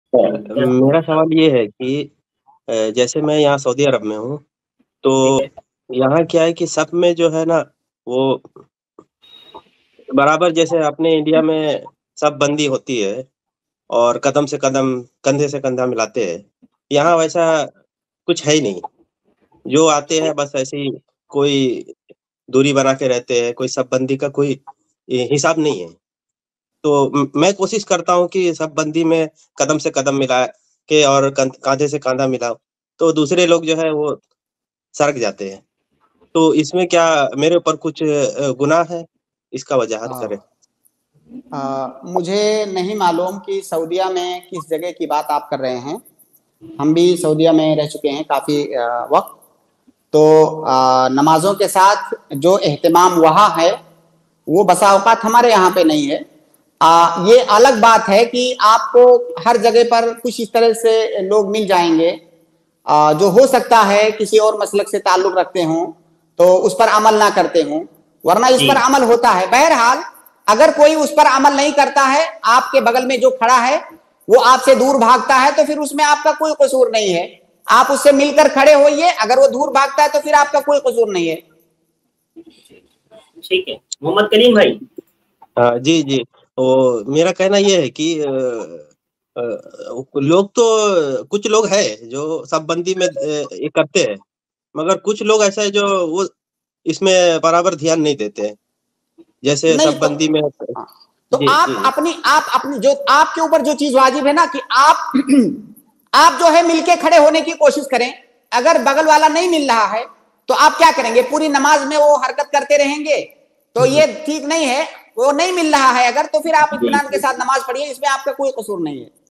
Namaz Mein Agar Paas Wala Shakhs Qadam Se Qadam Na Milaye To Kya Karein? by Dr. Naseem Madani
Namaz Mein Agar Paas Wala Shakhs Qadam Se Qadam Na Milaye To Kya Karein? by Dr. Mohammad Naseem Madani iPlus TV Deeni Q&A

UPI : donateilm@icici